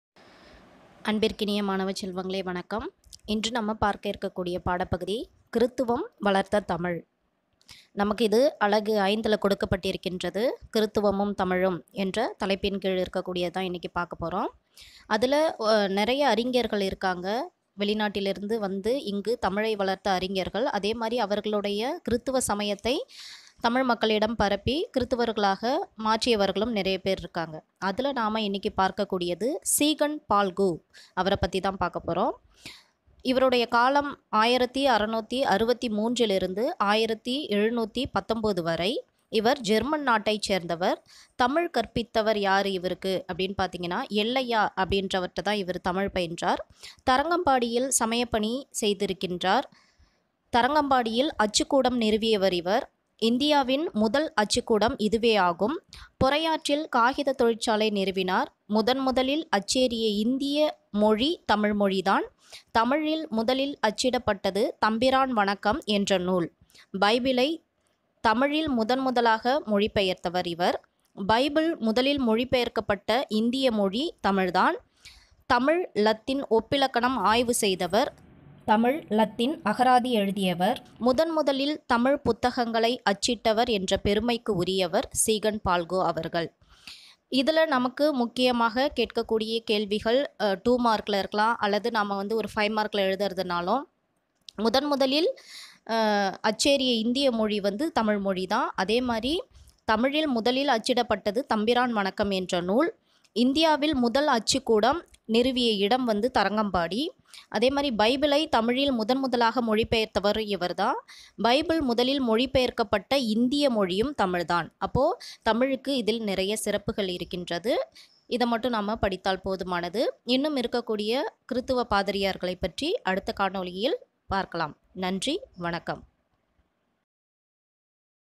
கிறித்துவம் வளர்த்த தமிழ் - சீகன்பால்கு/பொதுத்தமிழ் (அலகு 5)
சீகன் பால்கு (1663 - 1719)
ஜெர்மன் நாட்டவர்
தமிழ் கற்பித்தவர் எல்லயா
தரங்கம்பாடியில் சமயப் பணி செய்தார்
தரங்கம்பாடியில் அச்சுக்கூடம் நிறுவினார்.
இந்தியாவின் முதல் அச்சுக்கூடம் இதுவே ஆகும்.
பொறையாற்றில் காகிதத் தொழிற்சாலை நிறுவினார்.
முதன்முதலில் அச்சேறிய இந்திய மொழி தமிழ்.
தமிழில் முதலில் அச்சிடப்பட்டது 'தம்பிரான் வணக்கம்' என்ற நூல்.
பைபிளைத் தமிழில் முதன்முதலாக மொழிபெயர்த்தவர்.
பைபிள் முதலில் மொழிபெயர்க்கப்பட்ட இந்திய மொழி தமிழ்.
தமிழ் இலத்தீன் ஒப்பிலக்கணம் ஆய்வு செய்தவர்.
தமிழ் இலத்தீன் அகராதி எழுதியவர்.
முதன் முதலில் தமிழ் புத்தகங்களை அச்சிட்டவர் என்ற பெருமைக்கு உரியவர்.